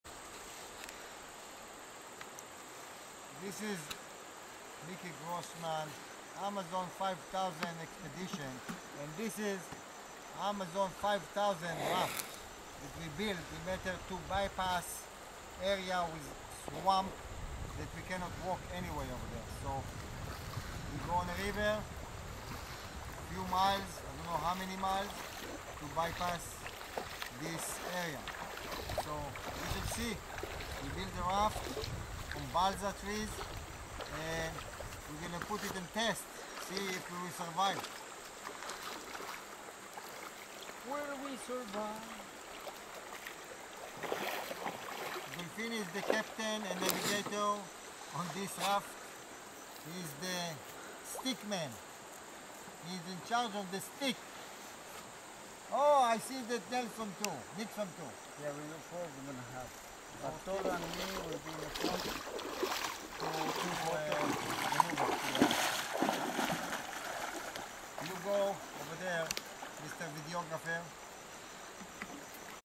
Building a raft made from balsa trees
Building a raft made from balsa trees In order to bypass endless swamp areas in the Amazon jungle